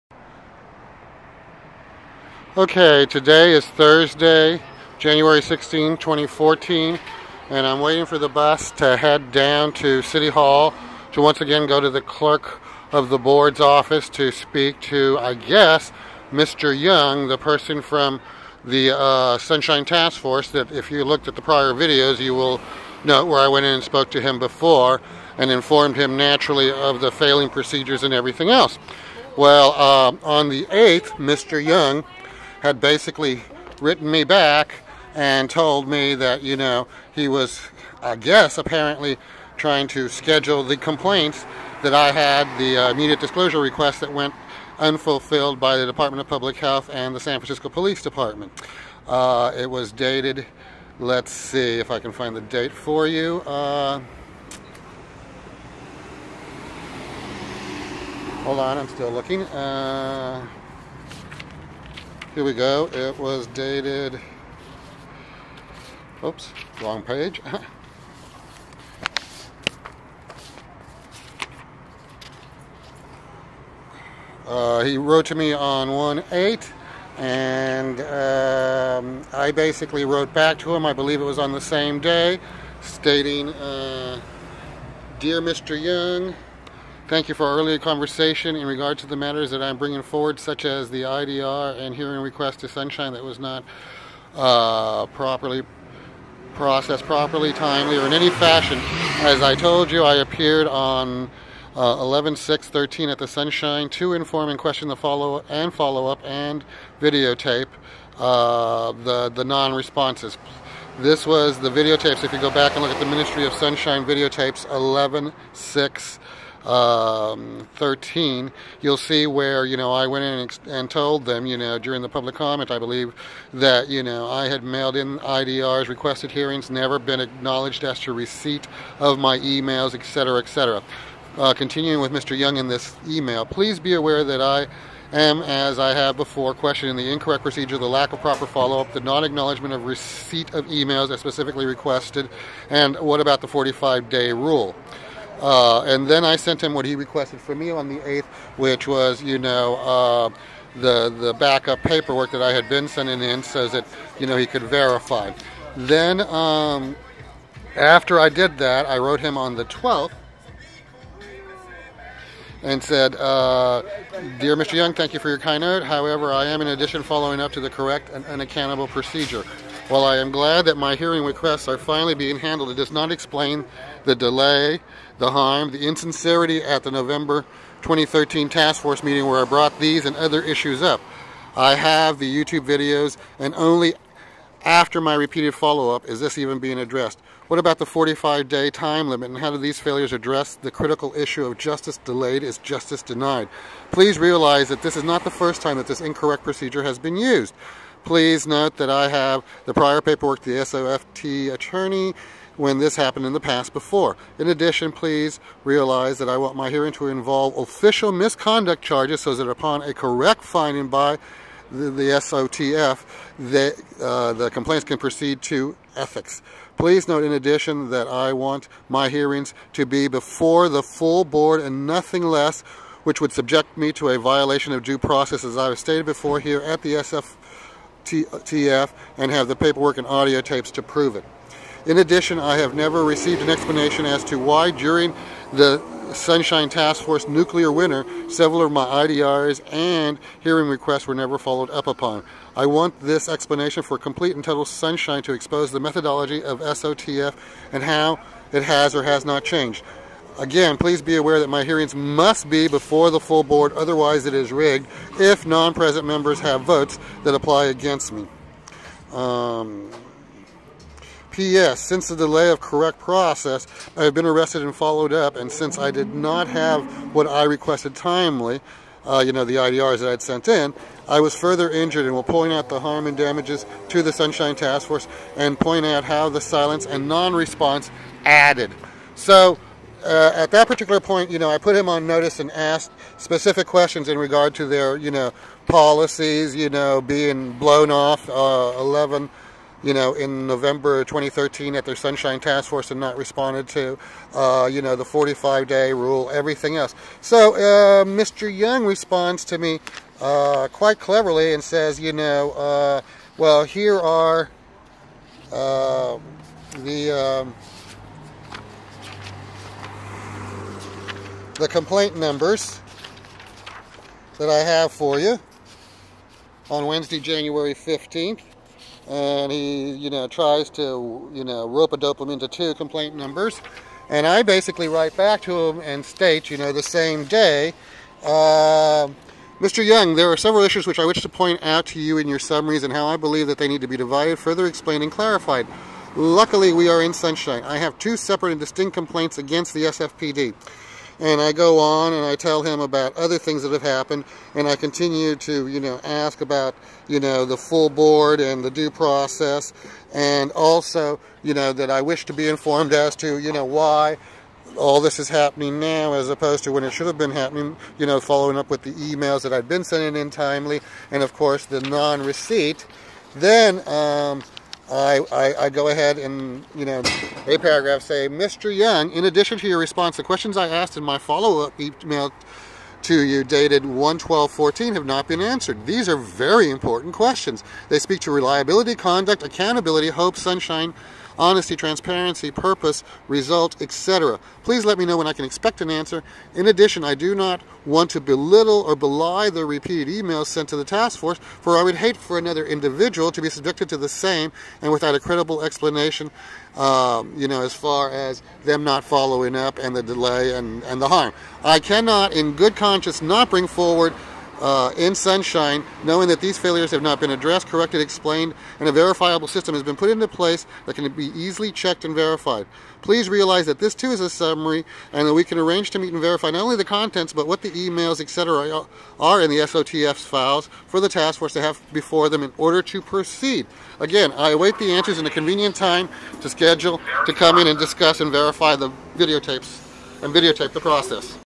011614p1 Ministry of Sunshine - IDR's & Hearing Requests; What consequence for SOTF's failures?
1/15/14
Mr. Garza:
                I have review the e-mail complaints and created a short summary (attached).   Please let me know if you have any comments and keep in mind this is only a summary.   Your original email complaint will be placed into the file for SOTF review.
                In the future, please clearly state your complaint to prevent confusion.  The related email can be placed into the file at supporting documents.   Thank you.
   14004  Complaint filed by Jason Garza, against the Police Department for allegedly violating the Sunshine Ordinance by not complying with an Immediate Disclosure Request in regards to videos, pictures and audio recordings from the Northern Station; copy of policies regarding video cameras and retention; contact information of ada/disability representative; and other related documents related to the referenced 5150 on 9/18/13.
   14005 Complaint filed by Jason Garza, against the Department of Public Health for allegedly violating the Sunshine Ordinance by not complying with an Immediate Disclosure Request in regards to Medical Screening Examinations Report and all documents related to the referenced 5150 on 9/18/2013.
Victor Young
Administrator
Sunshine Ordinance Task Force

1/12/14
Dear Mr. Young:
   Thank you for your kind note; however, I am still in addition following up to the INCORRECT and UNACCOUNTABLE procedure. While I am glad that my HEARING requests are FINALLY being handled ... it does NOT explain the delay, the HARM, the insincerity at the November 2013 Task Force meeting where I brought these and other issues up (I have the youtube videos) and ONLY after my repeated followup is this even being addressed.
   What about the 45 day time limit and how do these failures address the critical issue of JUSTICE delayed is JUSTICE denied. Please realize that this IS NOT the first time that this incorrect procedure has been used ( please note that I have prior paperwork to the SOTF attorney when this happened in the past before.) In addition please realize that I want my HEARINGS to involve "Official Misconduct" charges so that upon a CORRECT finding by SOTF ... they can proceed to ETHICS.
  Please note in addition that I want my HEARINGS to be before the FULL BOARD and anything less would subject me to a violation of DUE PROCESS as I have stated before here at SOTF and have the paperwork and audio tapes to prove.
   In addition, I have NEVER receive an explanation as to why DURING the SOTF's "nuclear winter" several of my IDR's an HEARING requests were never followed up upon. I want this explanation for complete and total SUNSHINE to expose the methodology of SOTF and how it has or HAS NOT changed.
   Again, BE AWARE that my HEARINGS must be before the FULL BOARD otherwise it is RIGGED if non-present members have votes that apply against me.

P.S. Since the delay of correct process, I have been arrested following up and since i did NOT have what was requested timely I was further injured and will point out the HARM and DAMAGES to the SOTF and point out how the silence and non-response added.
P.P.S. Have a nice day and GOD BLESS.